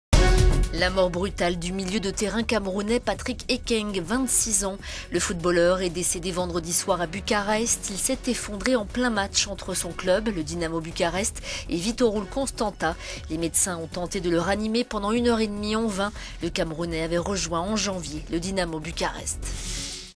Football : le Camerounais Patrick Ekeng décède en plein match
La mort brutale du milieu de terrain camerounais Patrick Ekeng, 26 ans. Le footballeur est décédé vendredi soir en Roumanie. Il s'est effondré en plein match entre son club le Dinamo Bucarest et Viitorul Constanta. Les médecins ont tenté de le ranimer pendant une heure et demie, en vain. Le Camerounais avait rejoint en janvier le Dinamo Bucarest qui occupe lors de la dernière journée des play-offs la quatrième position dans le Championnat de Roumanie.

"Je n'arrive pas à y croire. C'est un c…